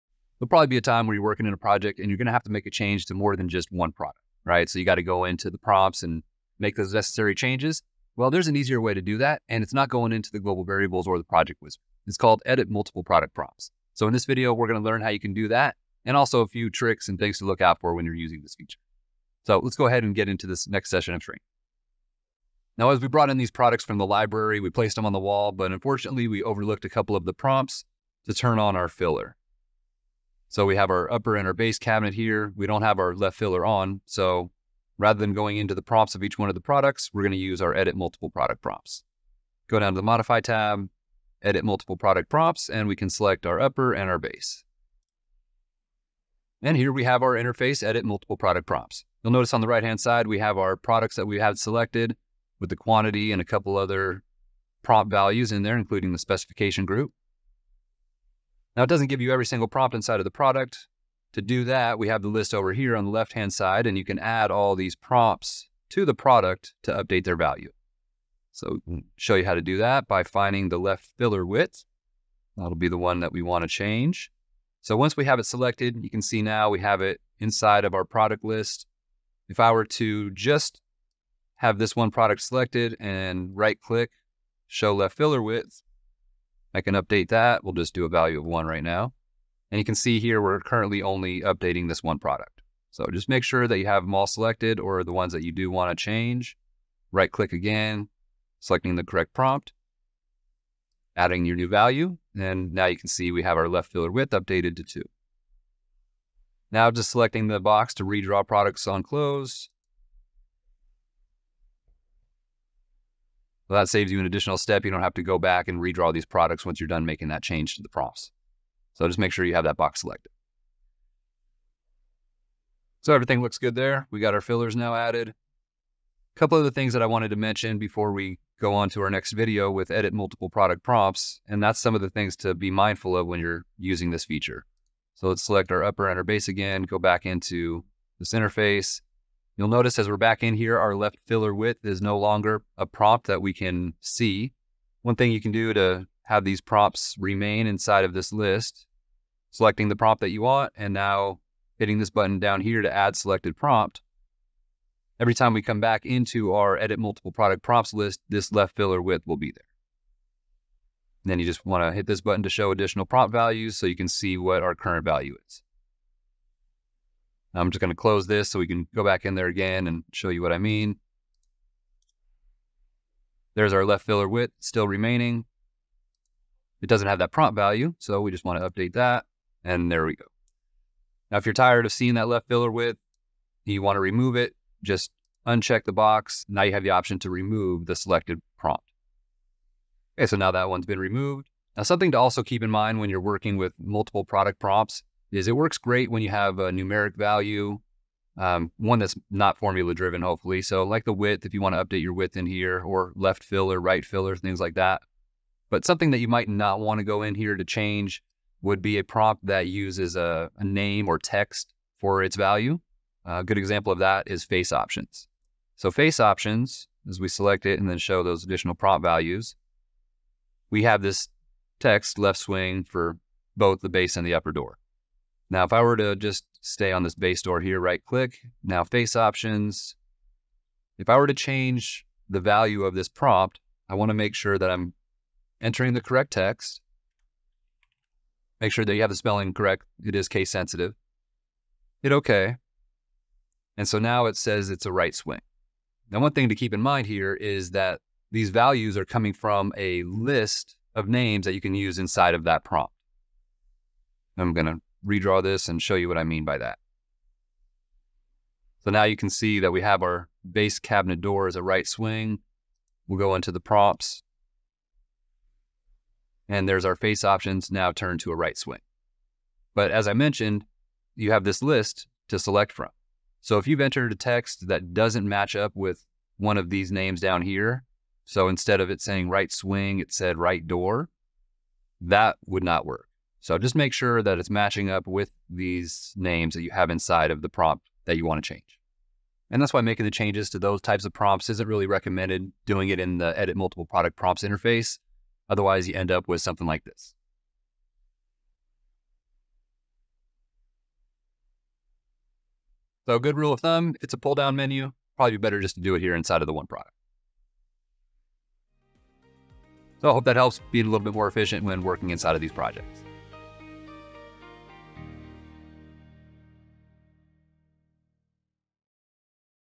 Editing Multiple Product Prompts
When managing your product library, it might be necessary to modify the prompts of various products to change specific values or configuration settings, such as your cabinet construction. Instead of editing each product individually in such a situation, the most efficient approach is to let the software assist you in making changes to multiple prompts simultaneously.

In this video, we demonstrate the effectiveness of the Edit Multiple Product Prompts feature and provide several tips, tricks, and suggestions to help you avoid mistakes to ensure you achieve the expected results.